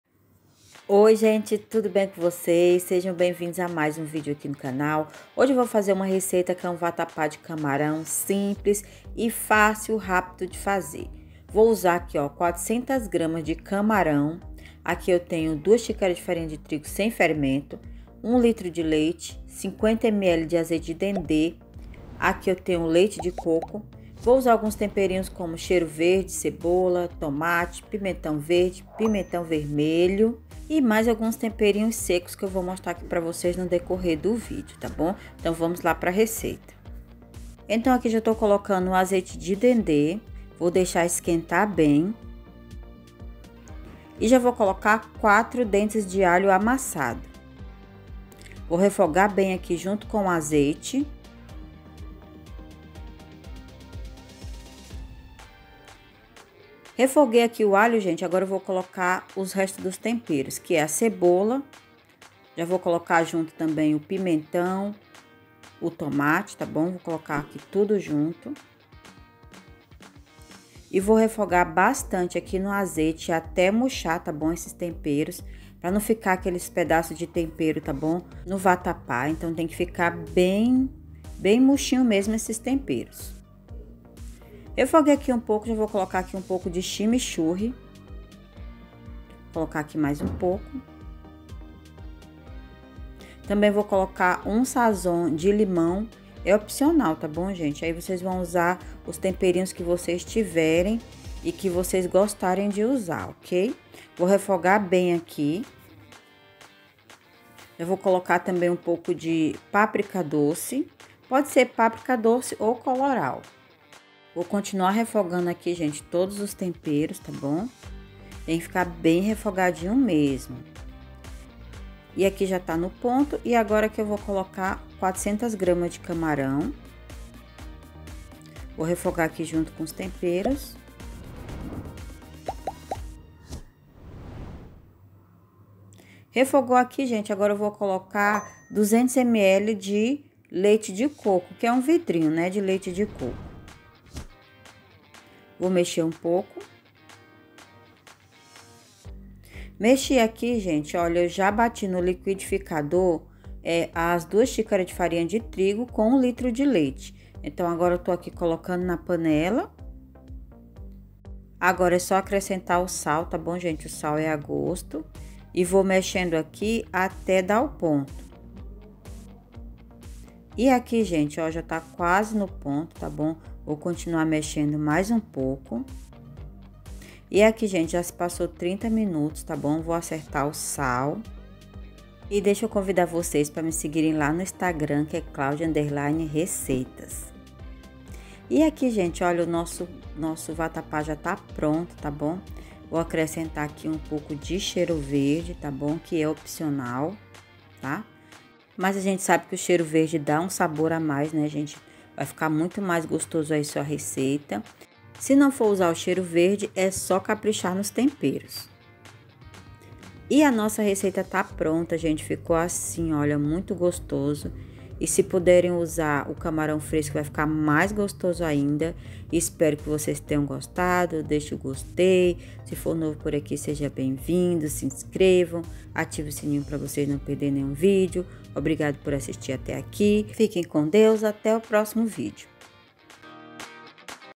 COMO FAZER VATAPA DE CAMARÃO SIMPLES
como fazer vatapa simples! amores mostrei como faço um vatapa de camarão delicioso !!
espero que gostem assista o vídeo até o final pra não perder os detalhes da receita!

Claudia_Receitas

ingredientes
400g de camarão
1litro de leite
2 xícaras de farinha de trigo( xícara de 120ml )
200ml de leite de coco
50ml de azeite de dende
temperos à gosto